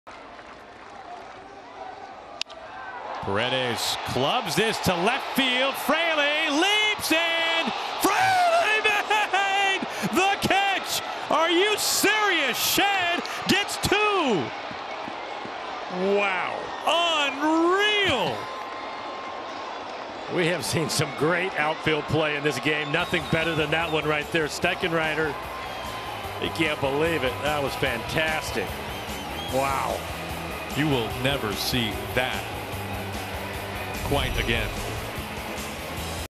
Jake Fraley robs potential walk-off Home Run: 6/9/2021
Jake Fraley makes a leaping catch to rob a potential walk-off homer from Isaac Paredes, then throws over to first for a double play.